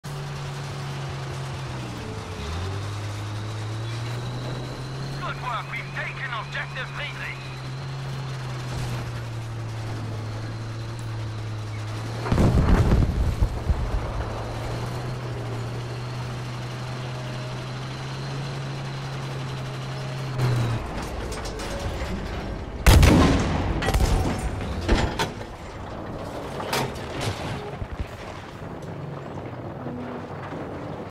TIGER TANK KILLS ANOTHER enemy BOMBER Plane!
Not easy shooting down an enemy bomber plane, especially as its flying right at me. Very difficult. Give Me a Knights Cross with Oak Leaves!